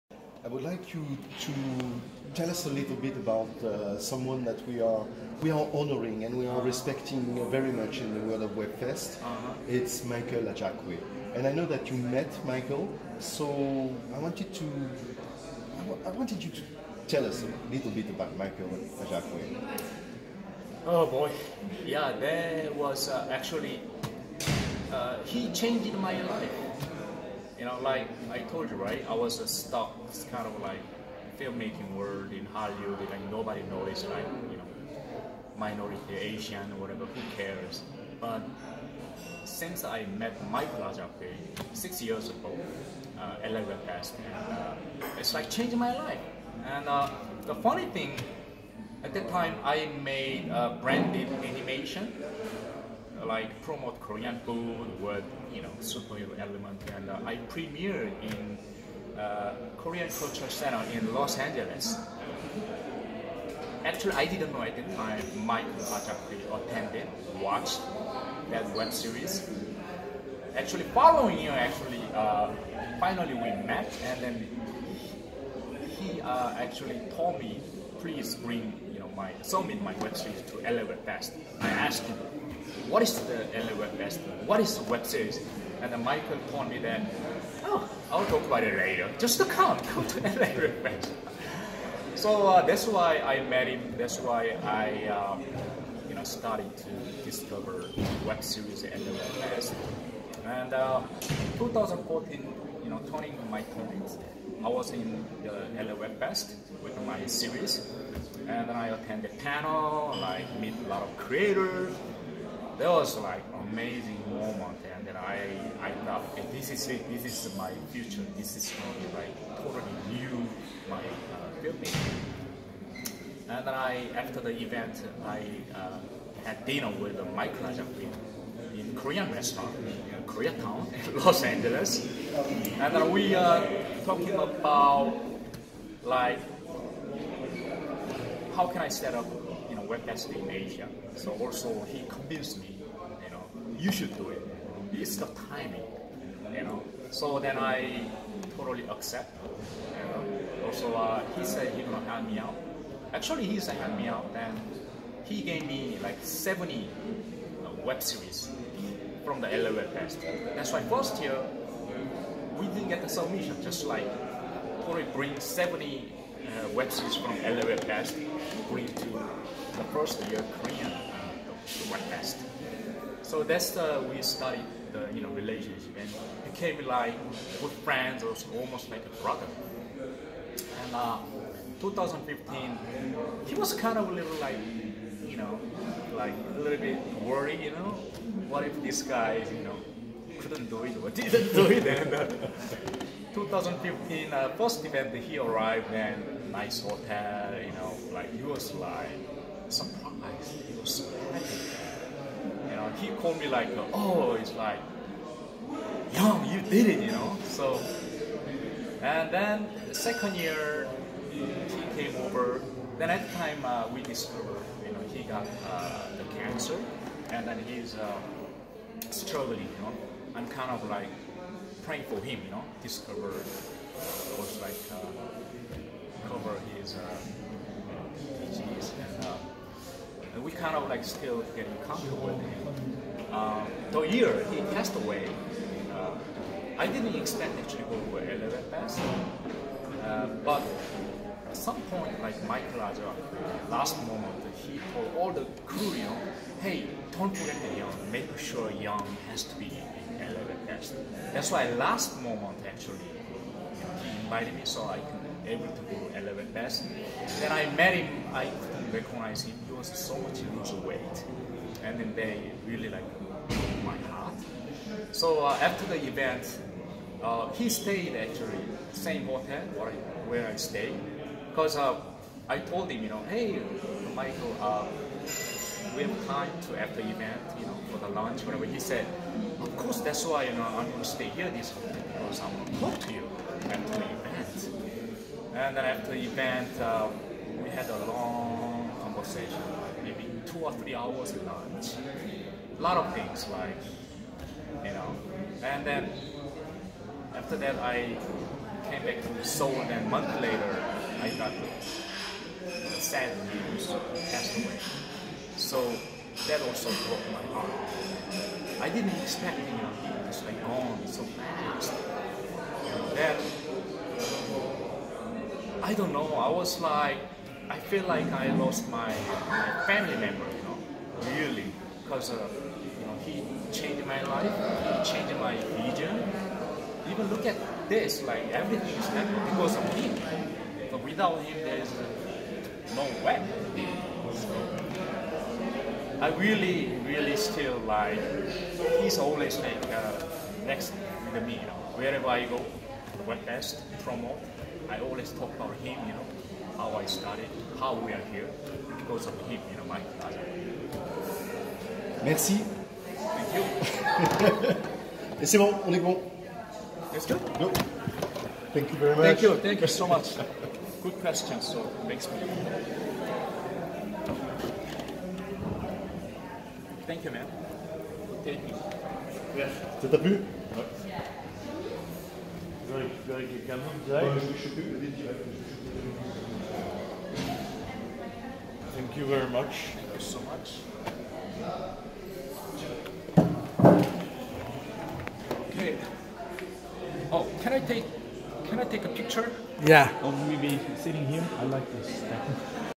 Young Man Kang talks about Michael Ajakwe, founder of LAwebfest.
Michael Ajakwe Jr. (October 12, 1965 - May 31, 2018) was a television, theatrical, and film writer and producer. The creator of the world's first web series festival and the founder of LAWEBFEST (Los Angeles Web Festival).